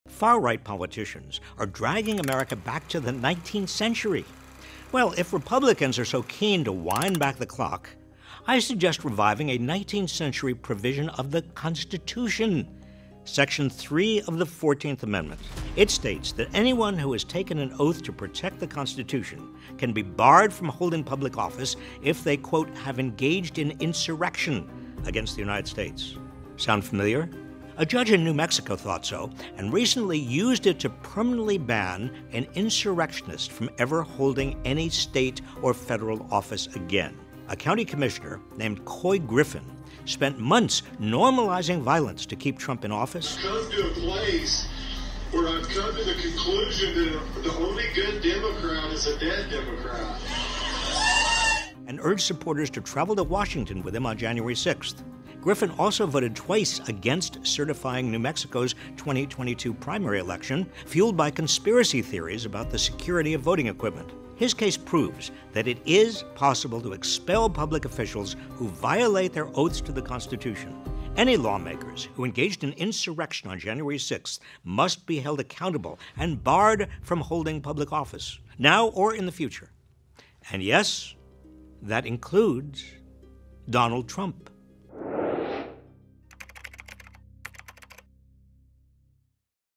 This One Thing May Stop Trump From Running For Re-Election | Robert Reich
Section 3 of the 14th Amendment bars anyone from office who has been found to have “engaged in insurrection" against the United States.

Yes, that includes Donald Trump.

He shouldn't be able to run for re-election.